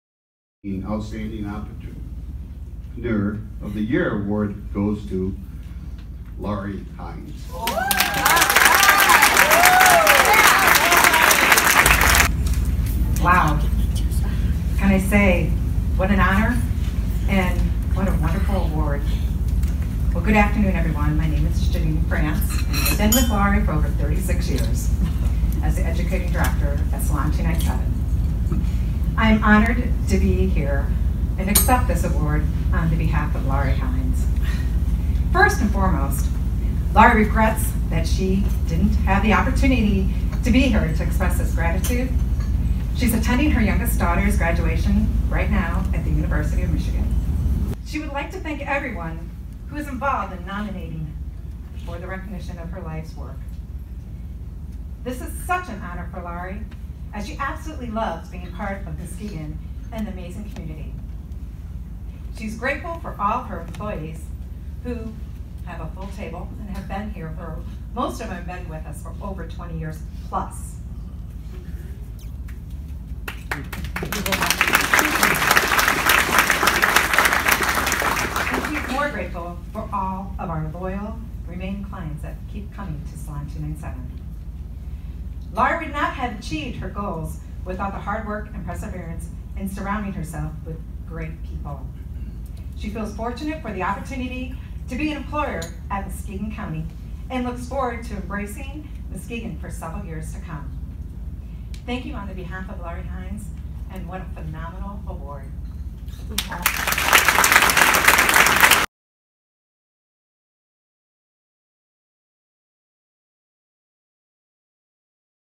Salon owner named Muskegon’s Outstanding Entrepreneur 2018
The Muskegon Lakeshore Chamber of Commerce recognized seven entrepreneurs on Friday, April 27, 2018, for their excellent work in the community. During the E Awards luncheon at the Muskegon Country Club, Lari Hines, owner of Salon 297 Hair Studio & Day Spa, the Outstanding Entrepreneur of 2018.